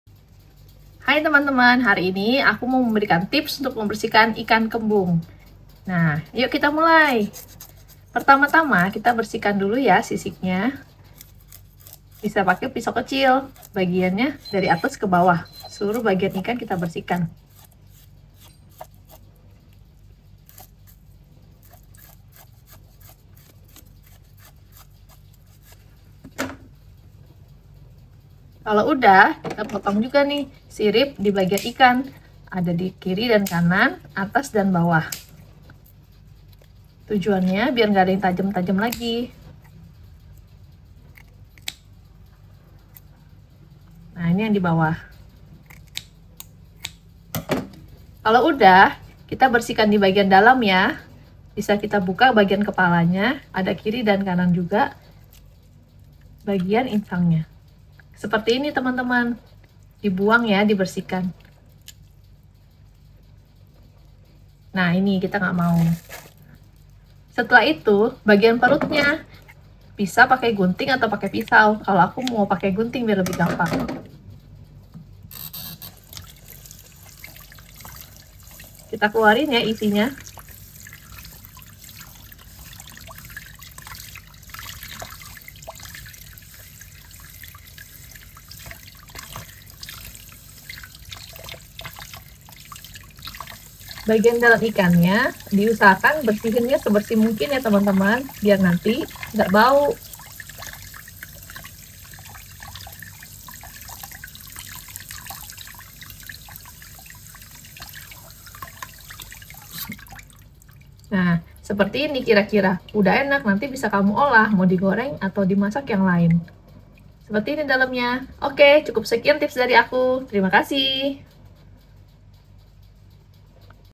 Cara Membersihkan Ikan Kembung Tips Belajar Langkah Memotong Sendiri Mudah Untuk Pemula
Apakah Anda pemula dalam dunia memasak ikan? Jangan khawatir, karena dalam video ini kami membawa Anda melalui tutorial praktis tentang cara membersihkan ikan kembung dengan mudah!

Dalam tutorial ini, kami akan menjelaskan setiap langkah secara rinci, sehingga bahkan pemula sekalipun dapat mengikuti dengan lancar. Kami akan membahas cara memotong, membersihkan, dan mempersiapkan ikan kembung dengan teknik yang sederhana namun efektif.

Dengan bimbingan langkah demi langkah kami, Anda akan mendapatkan hasil yang sempurna, ikan kembung yang siap untuk dimasak. Tidak hanya itu, kami juga akan memberikan tips berguna untuk memaksimalkan rasa ikan kembung ini, sehingga Anda dapat menyajikannya dengan bangga kepada keluarga dan teman-teman Anda.

Segera simak video ini dan jadilah ahli dalam membersihkan ikan kembung. Anda akan melihat bahwa memasak ikan sendiri itu tidaklah sulit! Selamat menikmati makanan lezat yang akan Anda buat sendiri setelah menonton tutorial ini.